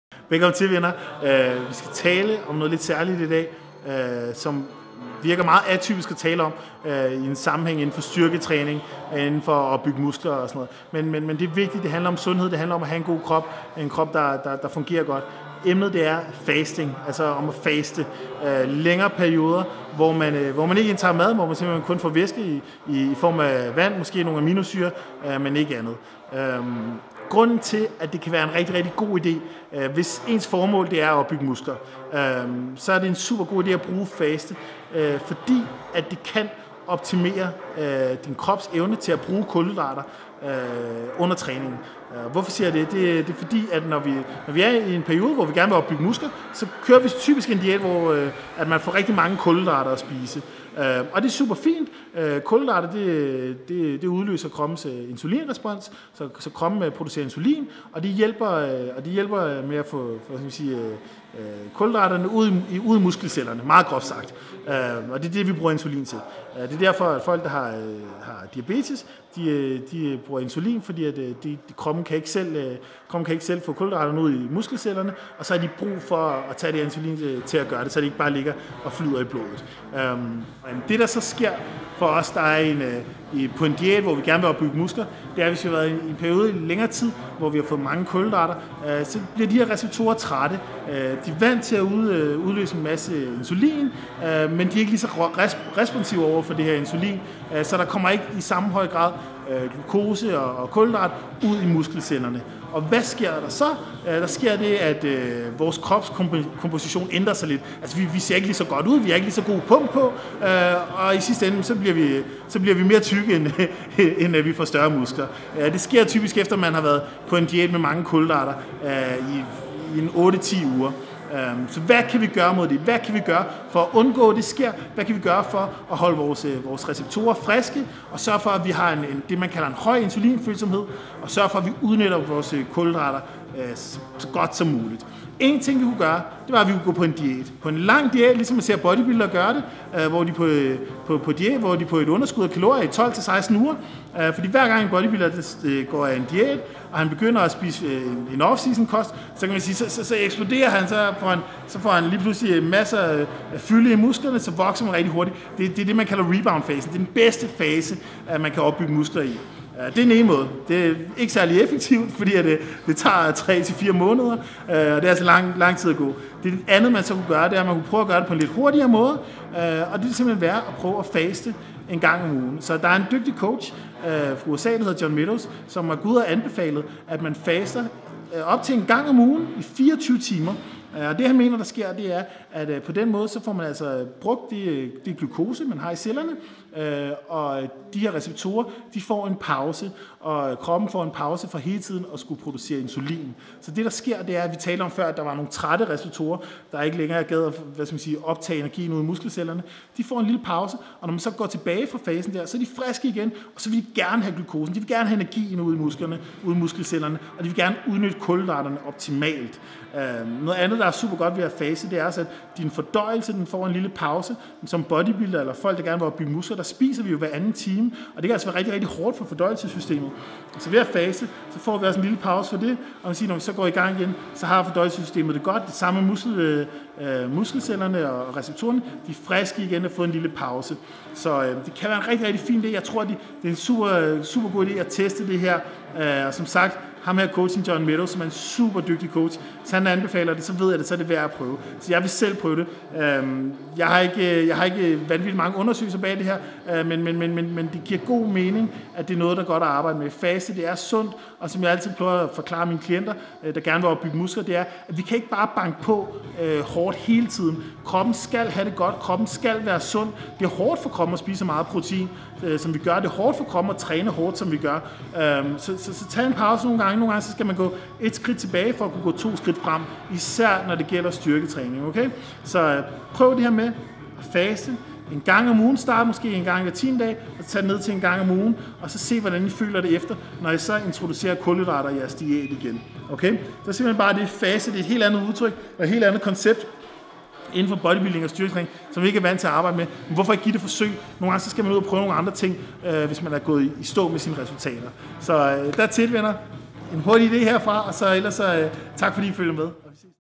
Faste! Et redskab til større muskler og mindre fedt%
Hvorfor er det så genialt at faste? Og hvorfor er jeg begyndt på det?